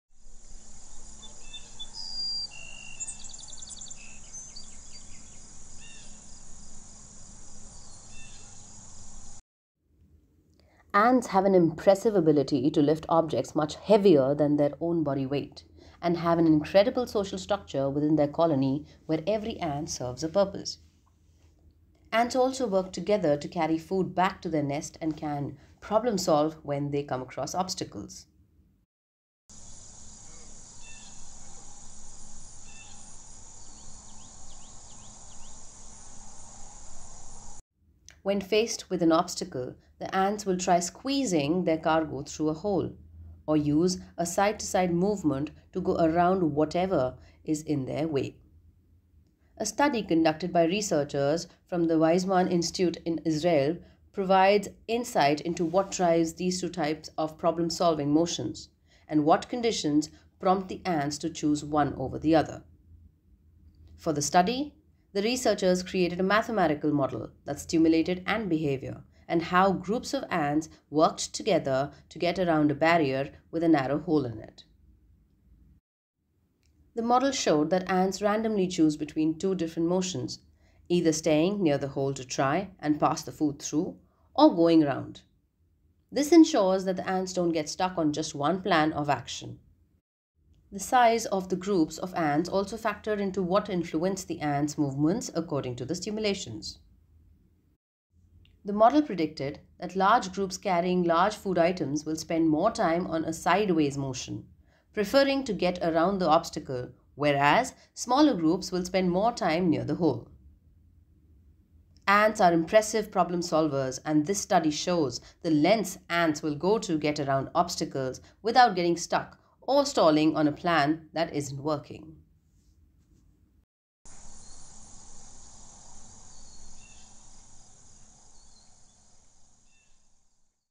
Ants - The Ultimate Teamwork Champions! | Ants working together | Nature Connection
Ants are the masters of collaboration, able to accomplish incredible feats through sheer teamwork. In this episode of Nature Connection, we'll delve into the fascinating world of these tiny creatures and discover the secrets behind their remarkable collective strength.

Witness the power of ants working together as a team:

Uncover the intricate communication system: Explore how ants use pheromones and physical touch to coordinate their movements and share information, allowing them to work together seamlessly.
Observe the impressive feats of strength: Witness how ants can carry objects many times their own weight, showcasing the power of collective action.
Discover the benefits of cooperation: Learn how teamwork allows ants to build complex structures, find food sources efficiently, and defend their colonies against threats.
Join us as we celebrate the amazing teamwork skills of ants and discover what we can learn from these tiny giants.